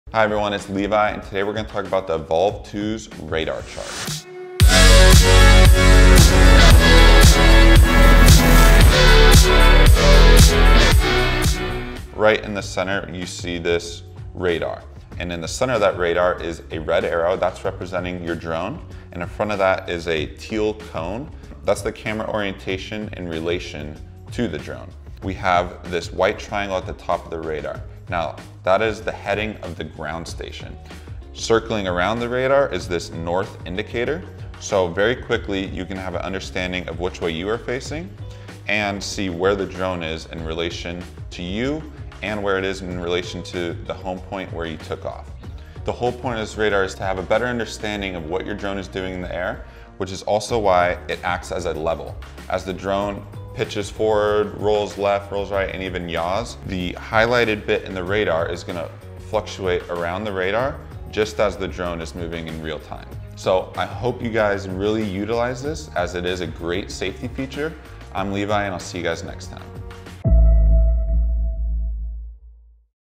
Inside the EVOLVE 2 | Radar Chart
The EVOLVE 2’s Radar Chart reveals a lot of important real-time information while in flight. Pilots can track the position and direction of the aircraft based on their physical location. The Radar Chart acts as a second set of eyes so you can worry less about the EVOLVE 2’s location and focus more on capturing amazing aerial video. Drone radar is one of the key functions of the Smart Pilot Assistant accessed through the Ground Station’s control panel.

What other flight functions are most useful to you?